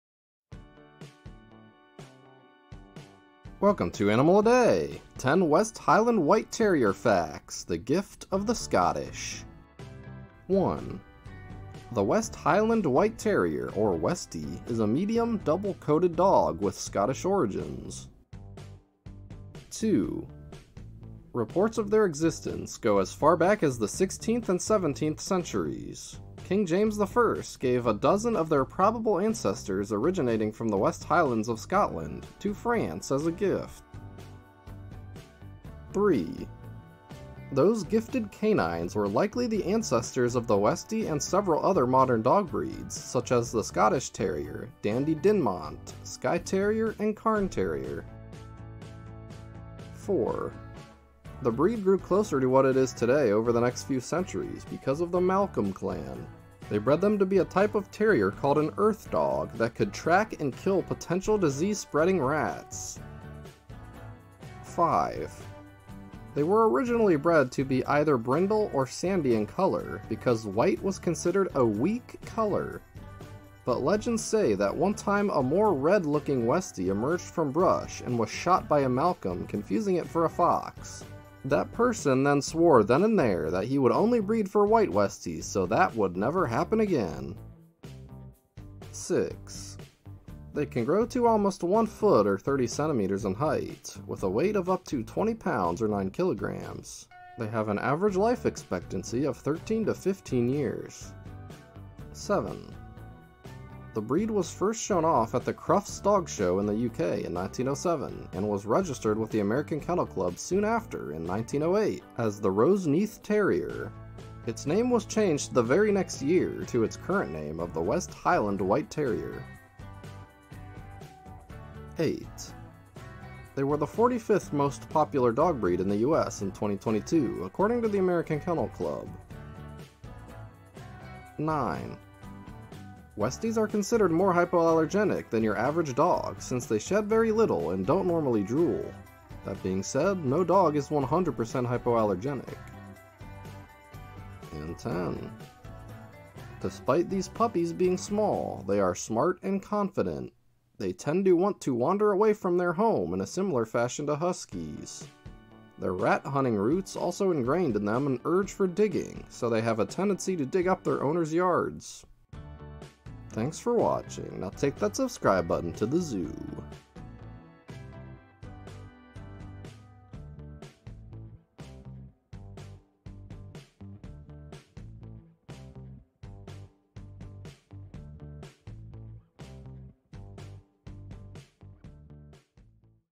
The Gift of the Scottish - West Highland White Terrier - Animal a Day
Today we learn about the pure white pup of the highlands, given as gifts by Scottish royalty, the White Highland White Terrier.